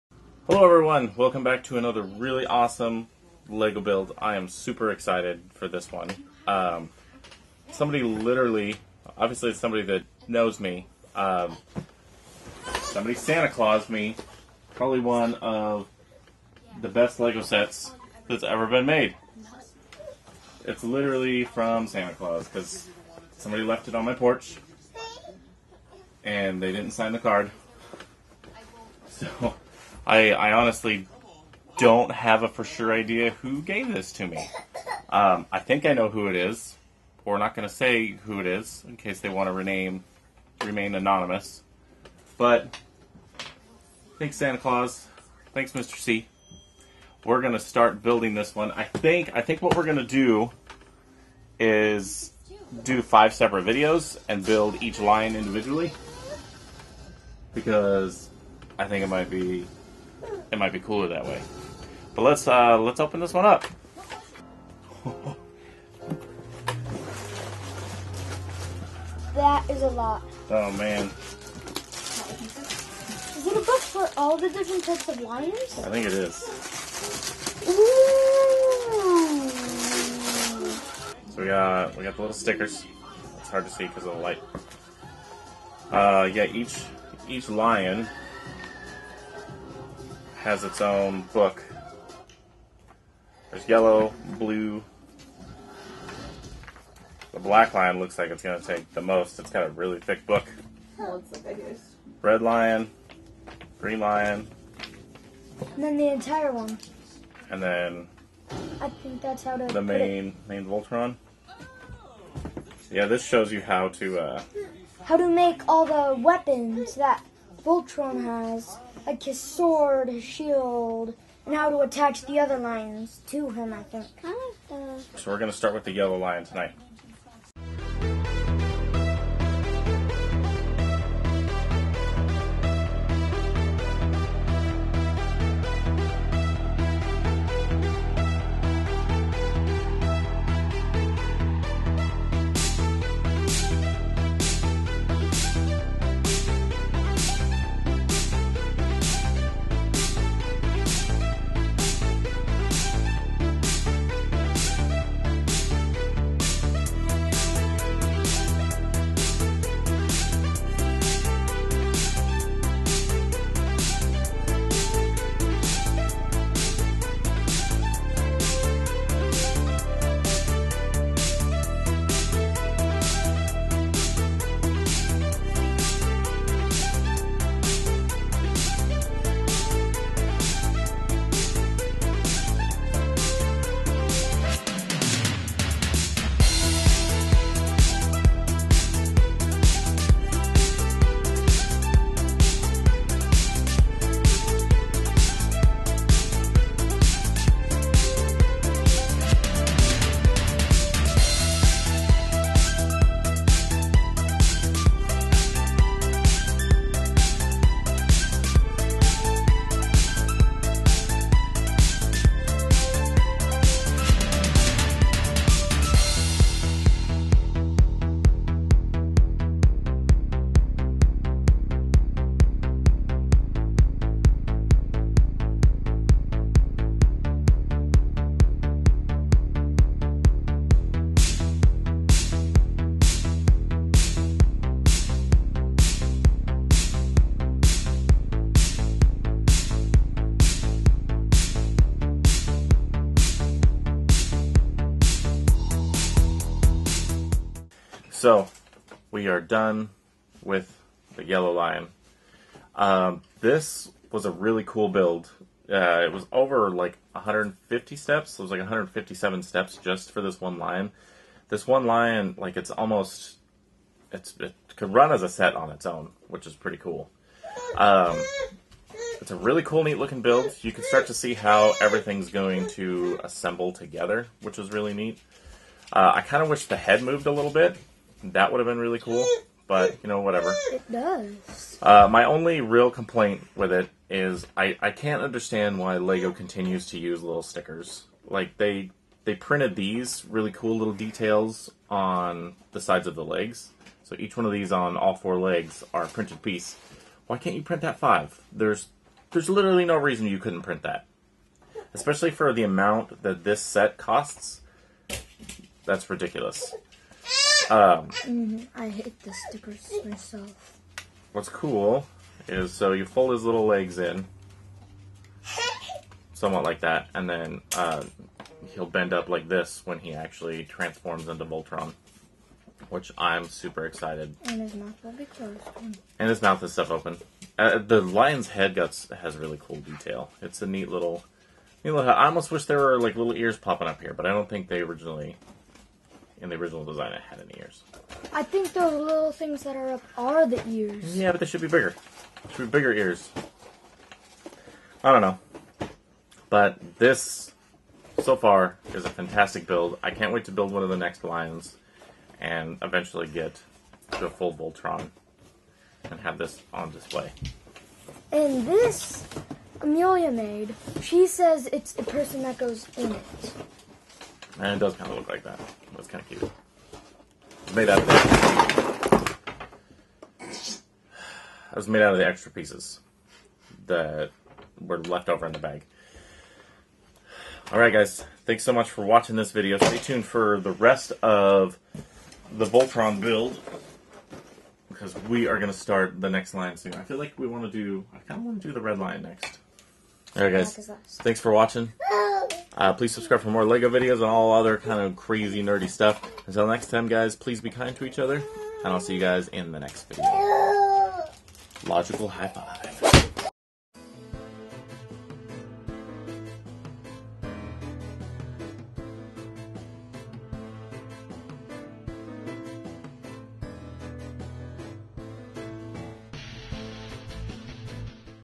Lego Voltron Yellow Lion Lego Vlog.
Santa brought me one of the best Lego sets ever!
Lego Voltron form Lego Ideas! The Yellow Lion Lego build vlog.